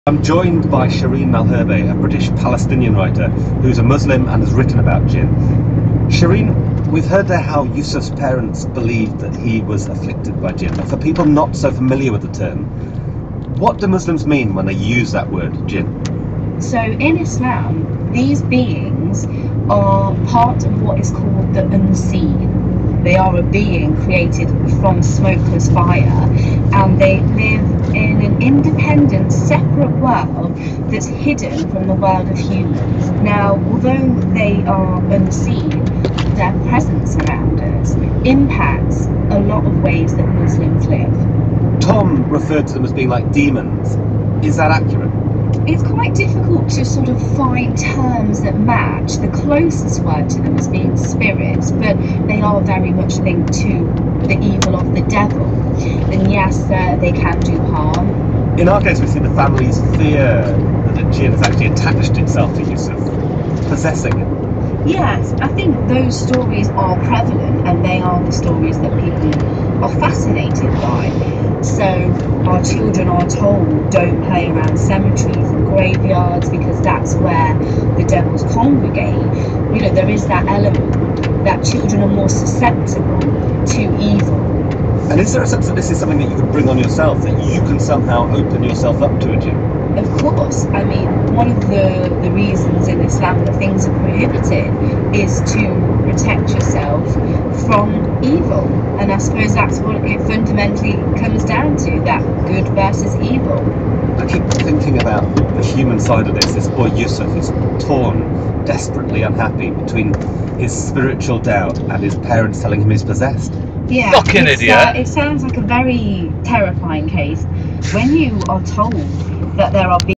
tractor driver pulls out
Watch the last 30 seconds.
I'm in a loaded 44 tonne liquid artic doing 50mph. I see him but don't expect him to just pull out.
I had to slow down fast from 50mph to about 10mph. As he pulled out I slowed down but didn't think I could stop, that's why I move to the right, to see if I can try to accelerate and get past him.